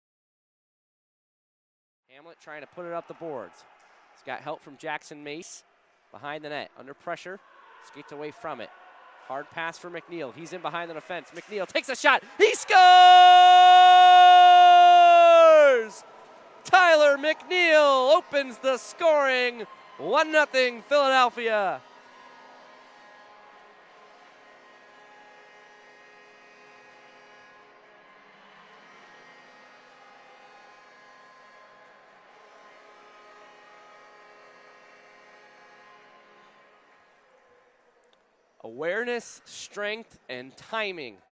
Tyler McNeil Goal USPHL NCDC Philadelphia Hockey Club vs Twin City Thunder
Tyler McNeil Goal USPHL NCDC Philadelphia Hockey Club vs Twin City Thunder. Hub City Tampa Florida. January 23, 2021.